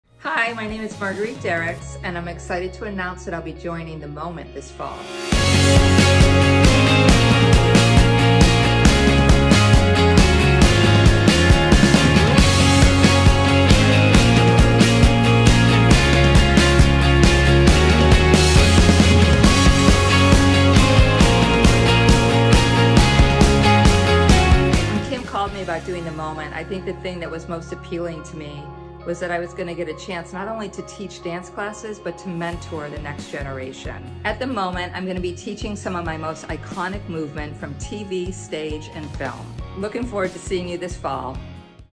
I'm Marguerite Derricks, and I'm excited to announce that I'll be joining The Moment this fall.
Kim called me about doing the moment. I think the thing that was most appealing to me was that I was going to get a chance not only to teach dance classes, but to mentor the next generation. At the Moment, I'm going to be teaching some of my most iconic movement from TV, stage and film. Looking forward to seeing you this fall.